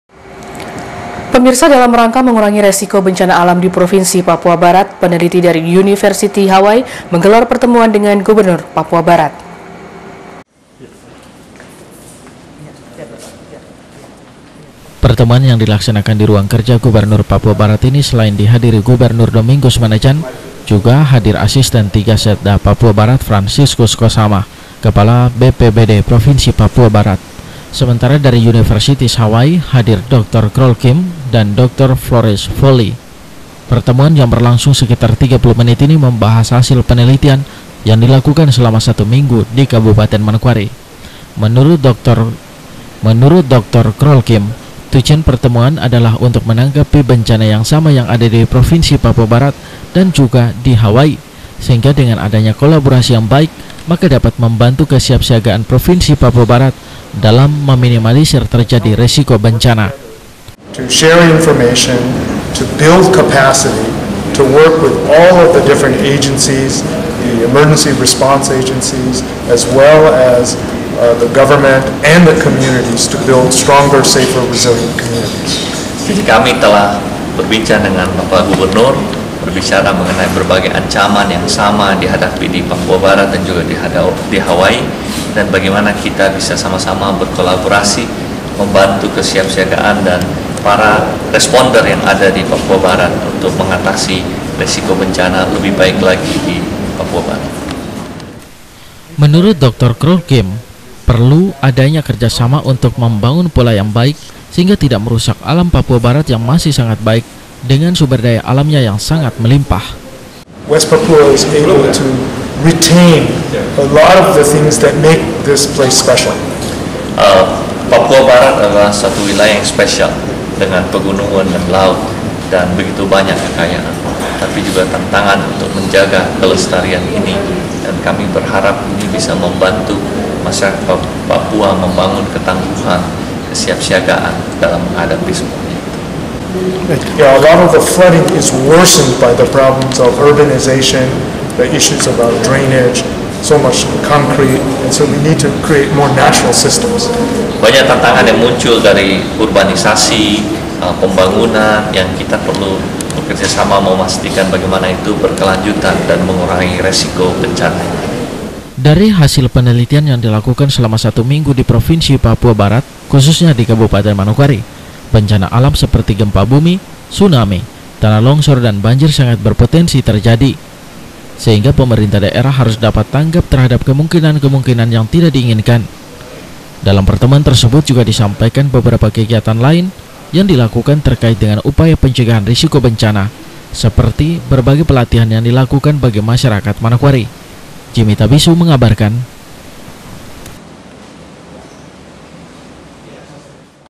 08 - KASUARI NEWS - PERTEMUAN GUBERNUR DENGAN UNIVERSITY HAWAI
Dalam Rangka Mengurangi Resiko Bencana Alam Di Provinsi Papua Barat, Peneliti Dari University Hawai Menggelar Pertemuan Dengan Gubernur Papua Barat.